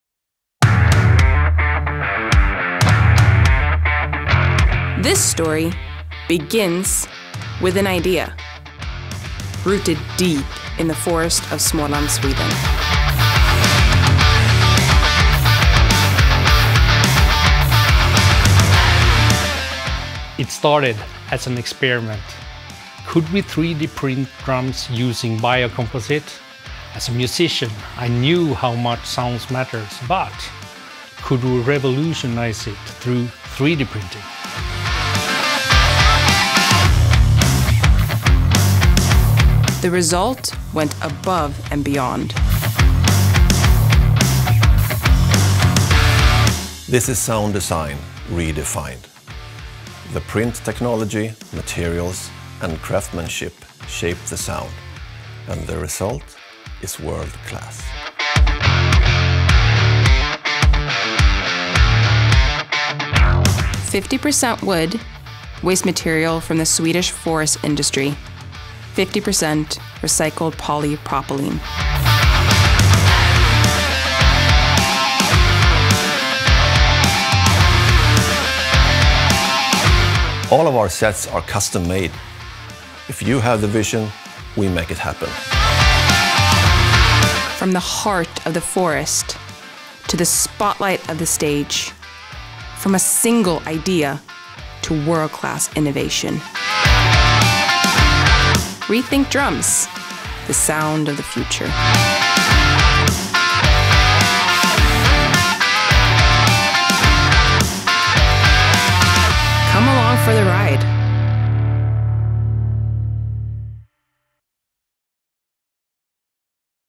Rethink Drums - Our Story
What happens when you mix Swedish forest materials, 3D printing, and a passion for sound?
The Story takes you behind the scenes of ReThink Drums — from a simple idea in Småland to a world-class instrument made from biocomposite.
🎧 Discover how we 3D print drums
🌱 Learn about sustainable sound design
🥁 Meet the people shaping the future of music